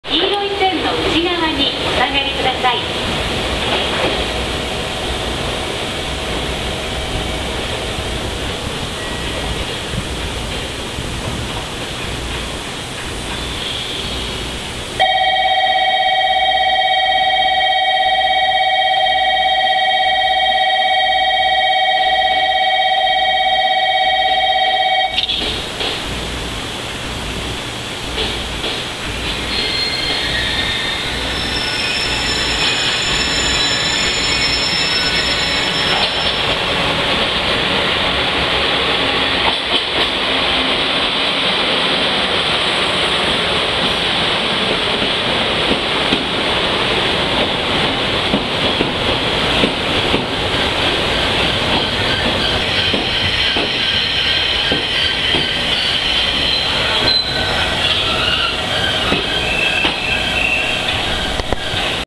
１０３系１５００番台 （３両編成）（試7894ﾚ） 【鳥栖駅・到着】
２０１１年０７月２７日に撮影しました。
Yahoo!ブログにて，鳥栖駅の到着時間を教えて貰ったので，急遽撮影に行きました。